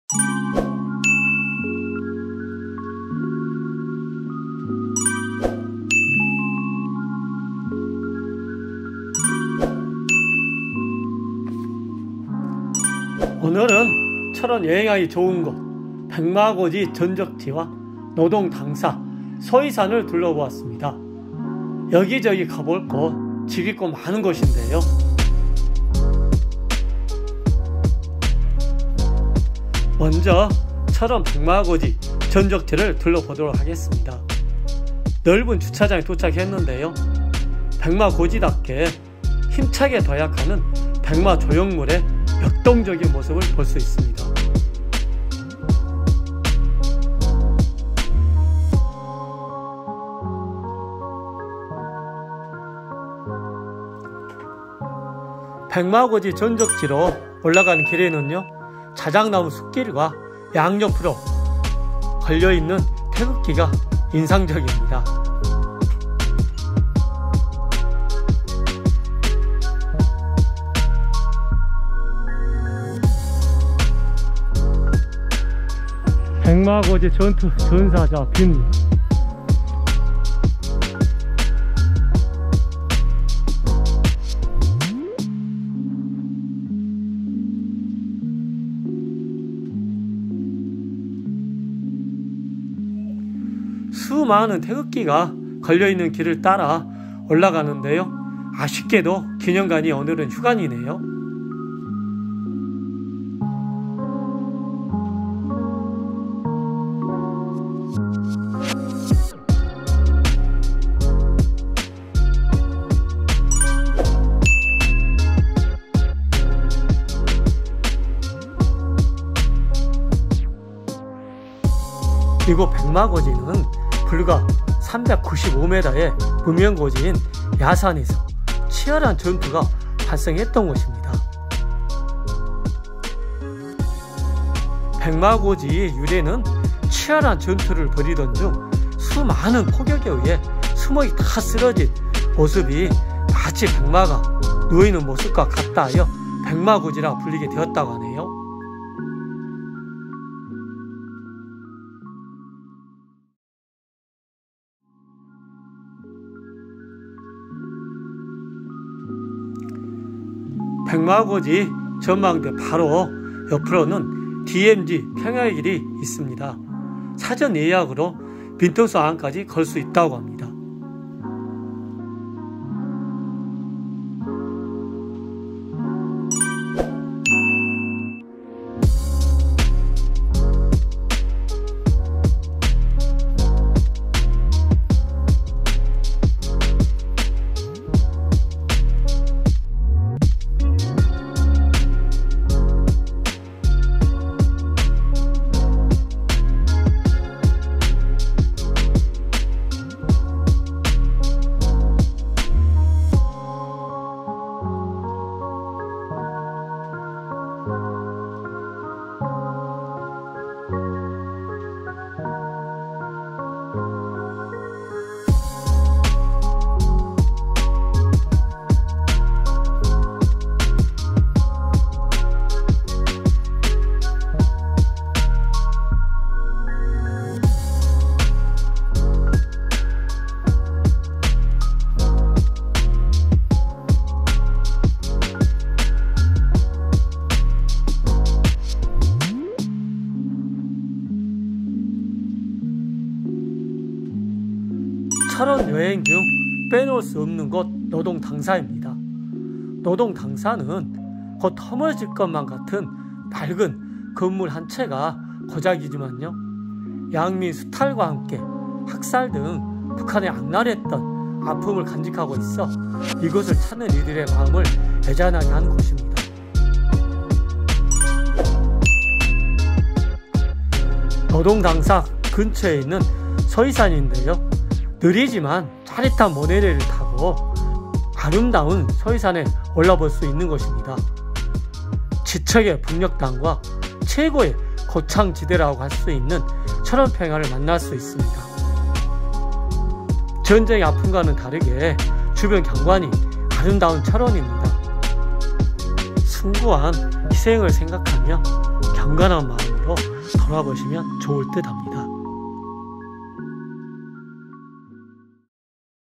철원가볼만한곳,소이산모노레일이용가능,가족여행지,철원평야와백마고지를한눈에볼수있는역사의현장.
철원여행하기좋은곳
백마고지전적기념관,노동당사,소이산등 역사의 현장을 한눈에 볼수 있는 가족여행지.